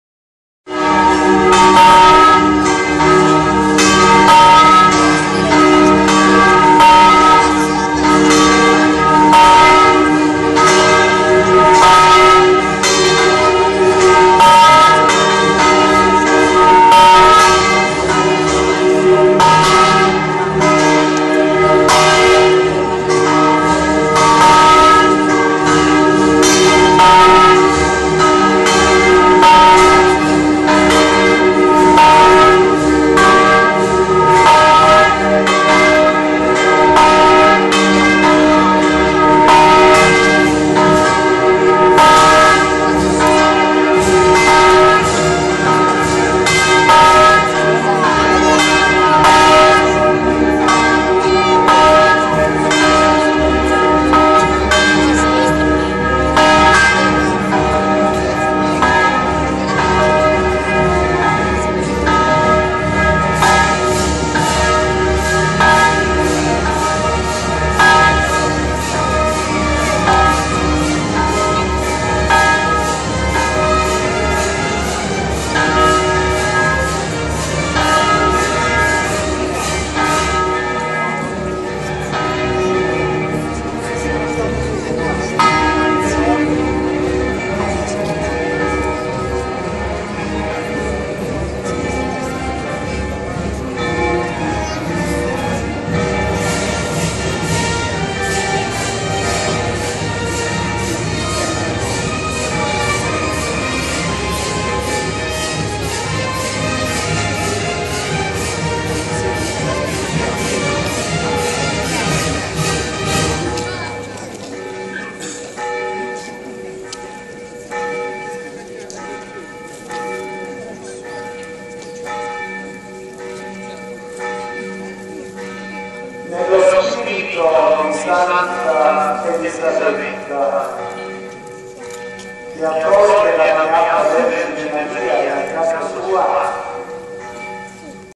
Festa di Mater Domini 2012 - 3 parte
La Processione fa sosta al centro della Cittá antica e si sofferma sul sagrato della Chiesa Madre per un momento di riflessione dell'Arciprete Mons. Angelo Argentiero